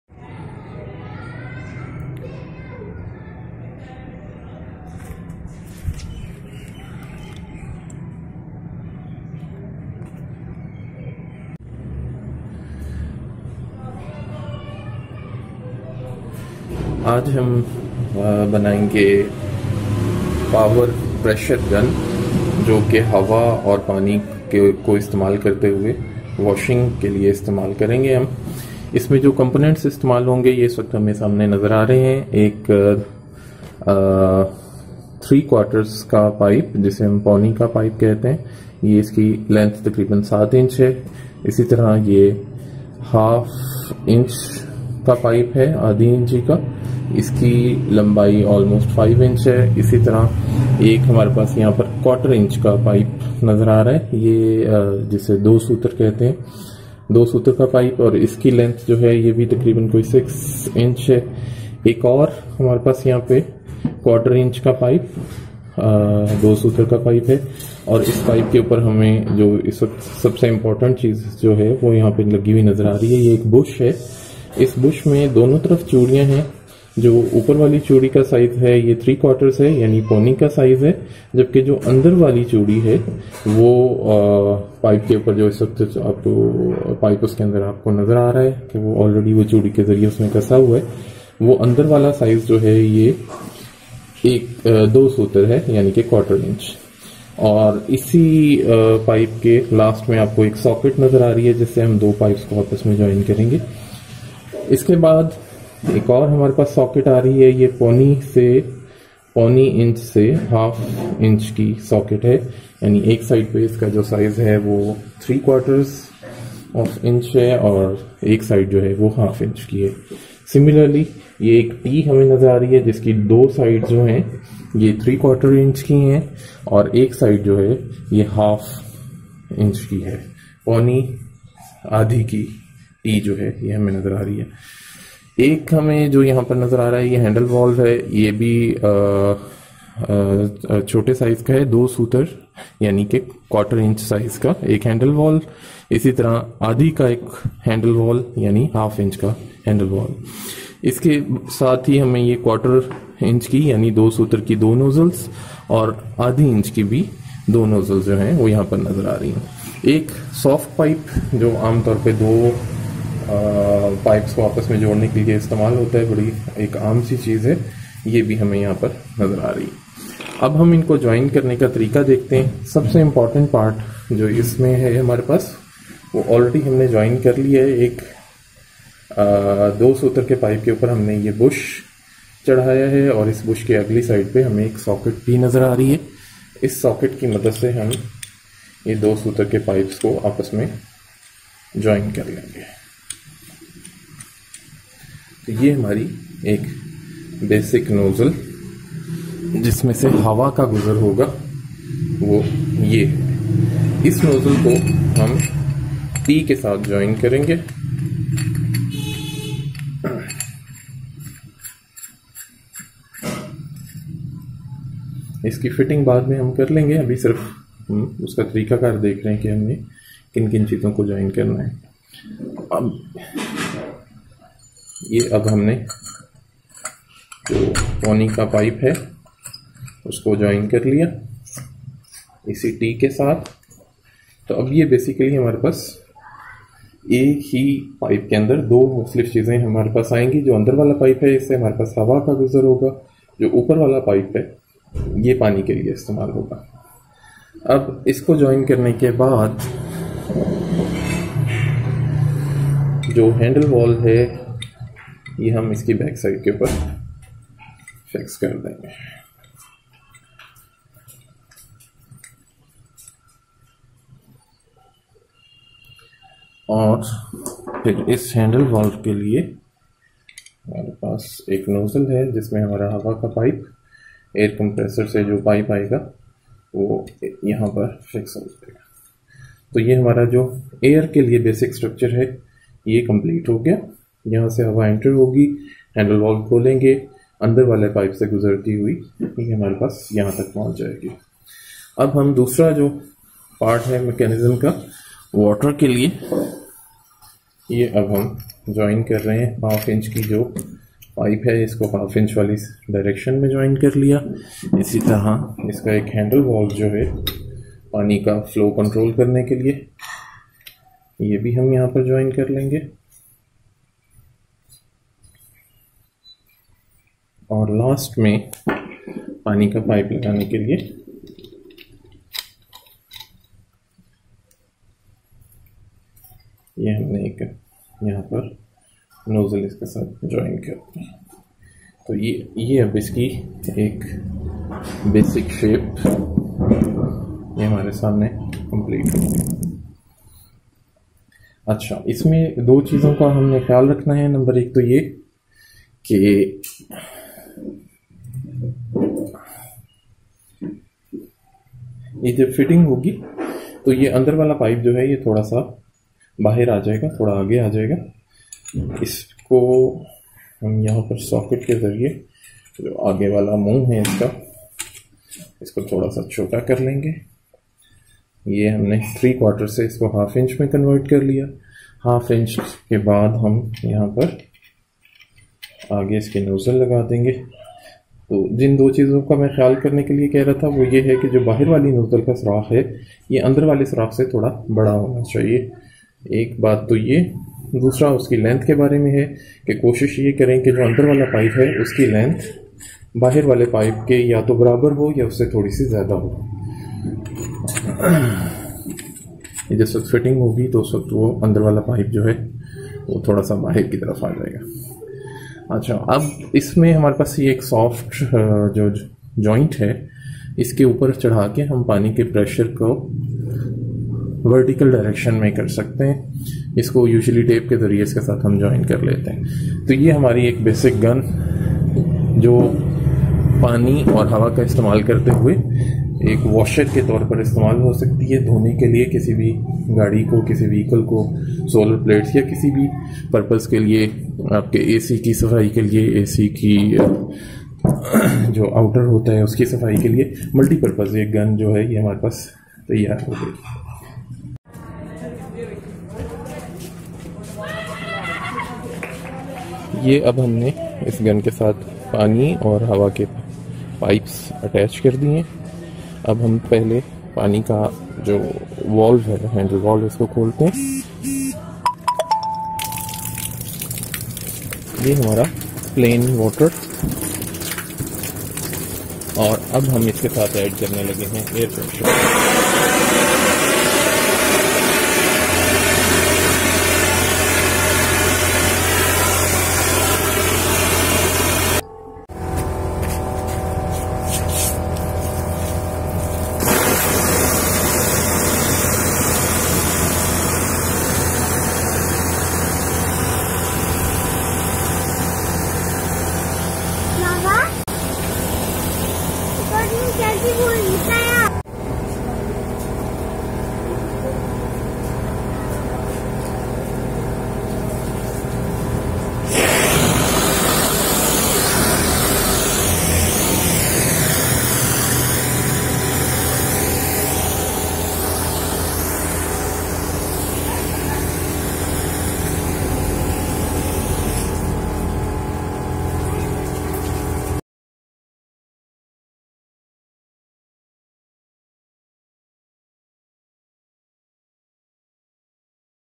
DIY Compressed Air Powered Pressure Washer Gun (Air + Water)
Combine power of Air with water to create power pressure washer gun. May be used for washing car, motorcycles, vehicles, solar panels, air conditioners, carpets, rugs, mats and all other materials which require pressure washing. Pressure of gun mostly depends upon power of Air Compressor. The more powerful compressor creates more washer pressure.